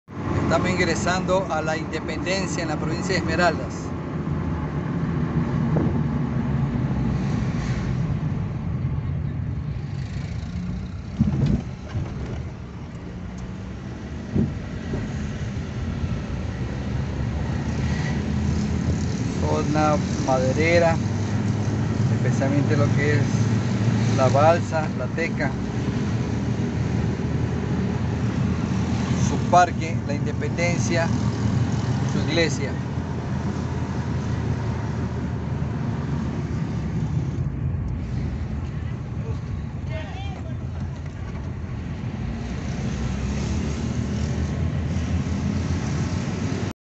Parroquia La Independencia de la Parroquia la Unión, Cantón Quininde.
Pertenencia de La Independencia no está en duda’

La Independencia es una jurisdicción territorial de 7.000 habitantes perteneciente a la parroquia La Unión, del cantón Quinindé, que por su ubicación geográfica está ubicada en el límite de la provincia de Esmeraldas con Pichincha y Santo Domingo de los Tsáchilas.